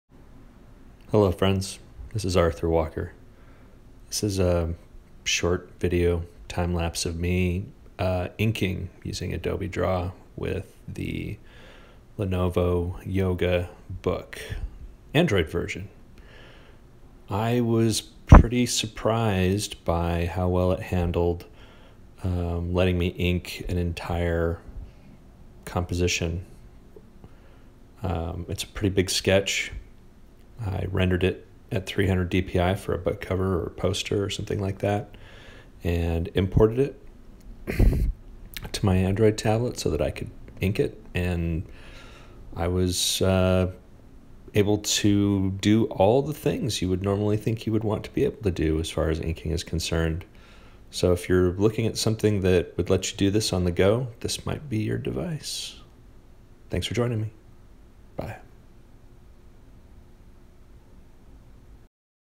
Lenovo Yoga Book Inking
Using a Lenovo Yoga Book, Android Version, and Adobe Draw to ink a sketch.